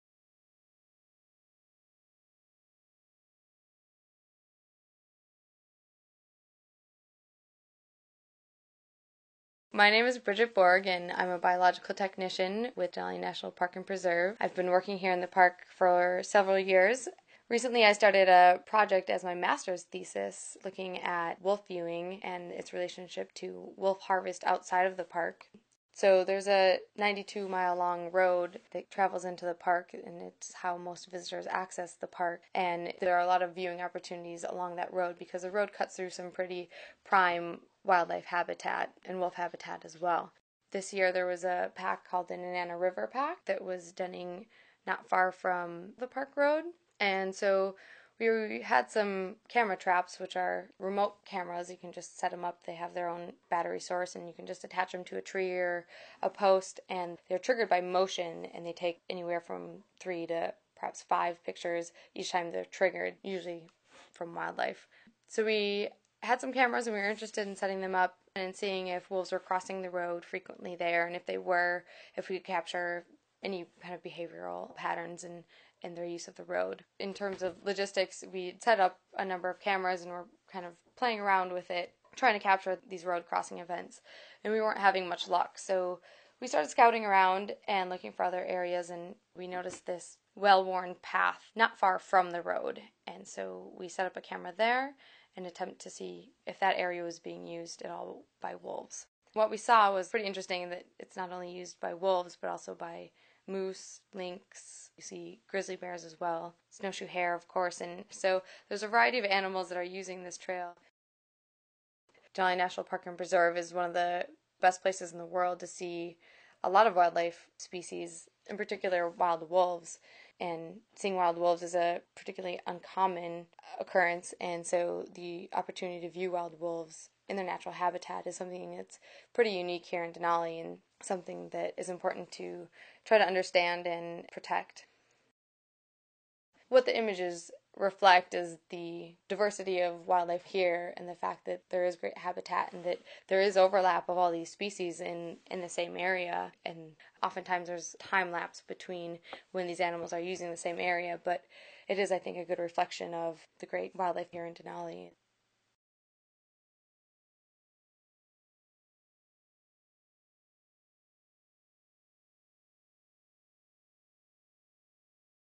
Studying Wolves with Camera Traps | Denali National Park, Alaska
Denali T-shirt, stickets, etc.
This video was recorded in Denali National Park and Preserve. Denali National Park and Preserve is located in Interior Alaska, centered on Mount Denali, the highest mountain in North America. The park and contiguous preserve encompasses more than 6 million acres (24,500 km2). The national preserve is 1,334,200 acres (5,430 km2). On December 2, 1980, a 2,146,580 acre (8,687 km2) Denali Wilderness was established within the park. Denali's landscape is a mix of forest at the lowest elevations, including deciduous taiga. The preserve is also home to tundra at middle elevations, and glaciers, rock, and snow at the highest elevations. The longest glacier is the Kahiltna Glacier. The park received 587,412 recreational visitors in 2016. Wintertime activities includes dog-sledding, cross-country skiing, and snowmachining.

Credit: NPS